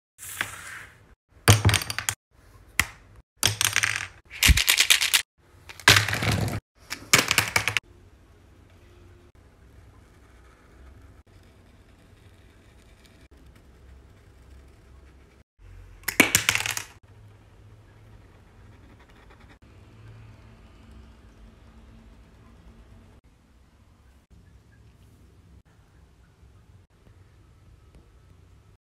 Drawing James Harden from The Brooklyn Nets with posca markers
Drawing James Harden from The Brooklyn Nets with posca markers on wood

Check out the other video’s!